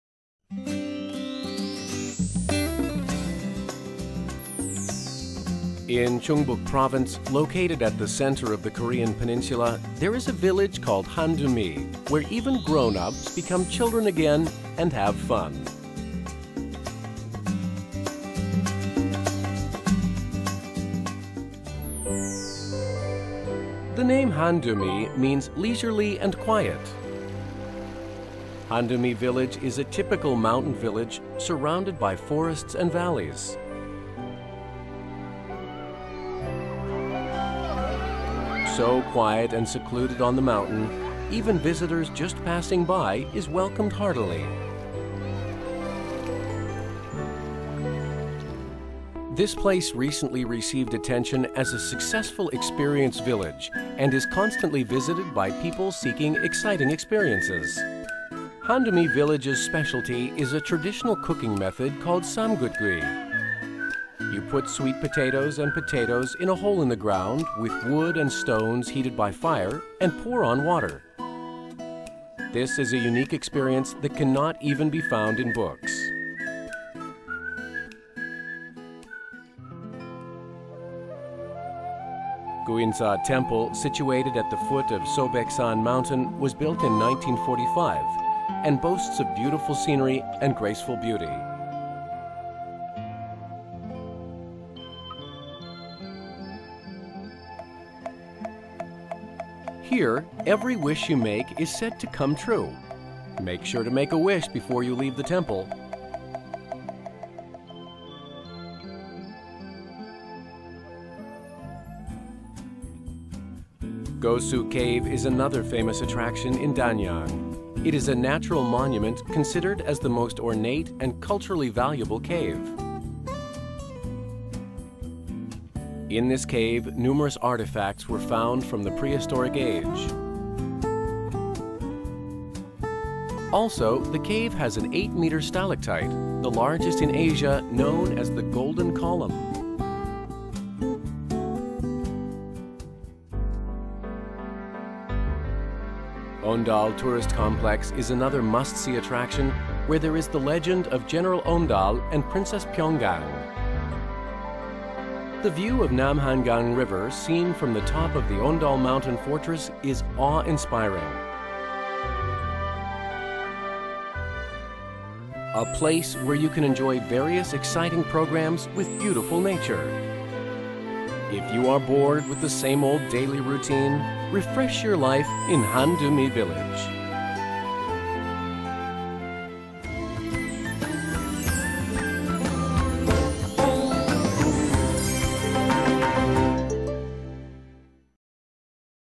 Rural-20: Handeuml Village
In Chungbuk Province, located at the center of the Korean peninsula, there is a village called Handeumi where even grown-ups become children again and have fun.
The name "Handeumi" means "leisurely and quiet." Handeumi Village is a typical mountain village, surrounded by beautiful forests and valleys. So quiet and secluded in the mountain, even visitors just passing by is welcomed heartily.
This place recently received attention as a successful experience village and is constantly visited by people seeking exciting experience. Andeumi Village's specialty is traditional cooking method called 'Samgutgui'! You put sweet potatoes and potatoes in the ground hole with woods and stones heated by the fire and pour water. This is a unique experience that cannot even be found in books.
Guinsa Temple, situated at the foot of Sobaeksan mountain, was built in 1945, and boasts of beautiful scenery and graceful beauty. Here, every wish you make is said to come true. Make sure to make a wish before you leave the temple.
Gosu Cave is another famous attraction in Danyang. It is a natural monument, considered as the most ornate and culturally valuable cave. In this cave, numerous artifacts were found from the prehistoric age. Also, the cave has a 8m stalactite, the largest in Asia, known as "The Golden Column."
Ondal Tourist Complex is another must-see attraction where there is the legend of General Ondal and Princess Pyeonggang. The view of Namhangang River seen from the top of the Ondal Mountain Fortress is awe-inspiring.
A place where you can enjoy various exciting programs with beautiful nature. If you are bored with same old daily routine, refresh your life in Handeumi Village.